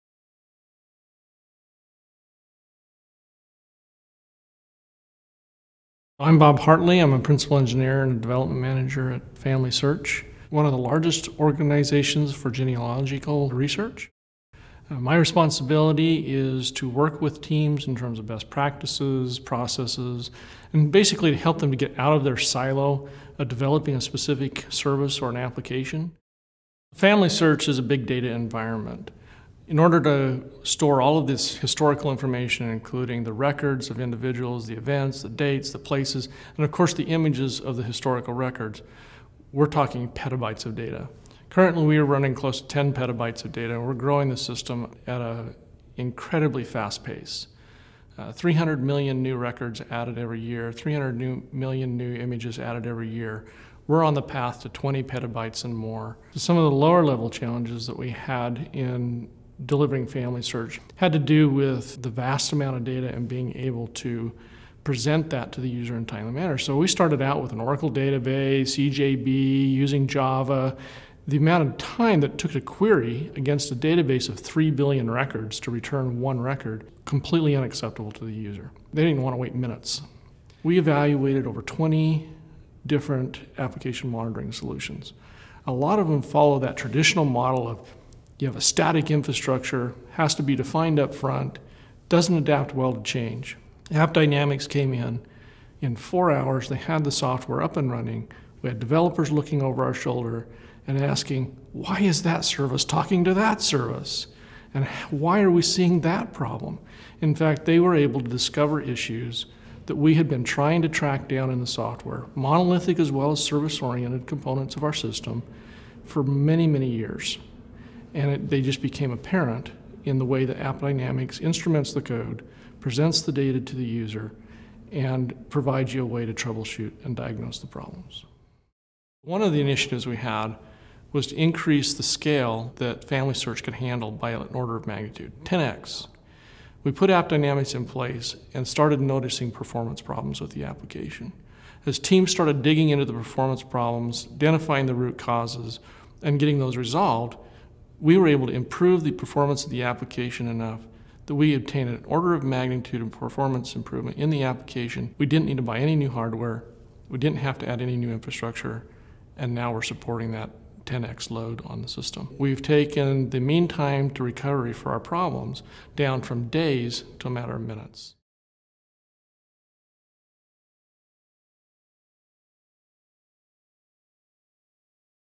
FamilySearch talks AppDynamics
Bob Hartley, Principal Engineer and Development Manager at FamilySearch, talks about how he used AppDynamics to optimize his big data applications and avoid purchasing additional infrastructure.